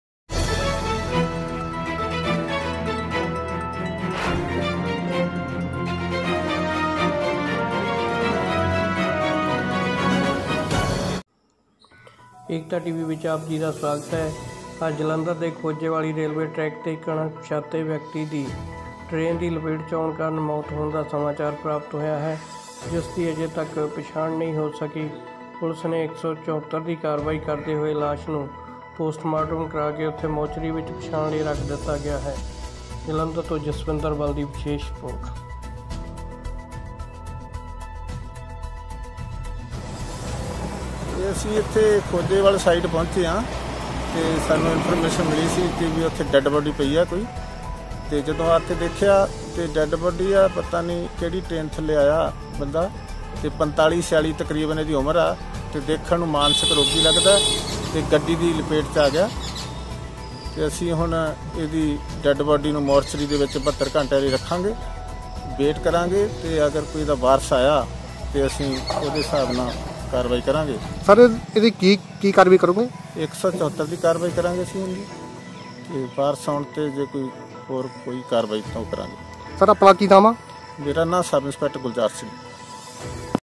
ਖੋਜੇਵਾਲ ਰੇਲਵੇ ਟਰੈਕ ਤੇ ਮਿਲੀ ਅਣਪਛਾਤੇ ਵਿਅਕਤੀ ਦੀ ਲਾਸ਼, ਮਿਰਤਕ ਦੀ ਨਹੀਂ ਹੋ ਸਕੀ ਪਹਿਚਾਣ।
ਸਾਡੇ ਚੈਨਲ ਤੇ ਖ਼ਬਰਾਂ ਅਤੇ ਇਸ਼ਤਿਹਾਰ ਦੇਣ ਲਈ ਸਾਡੇ ਨੰਬਰ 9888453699 ਤੇ ਸਪੰਰਕ ਕਰੋ।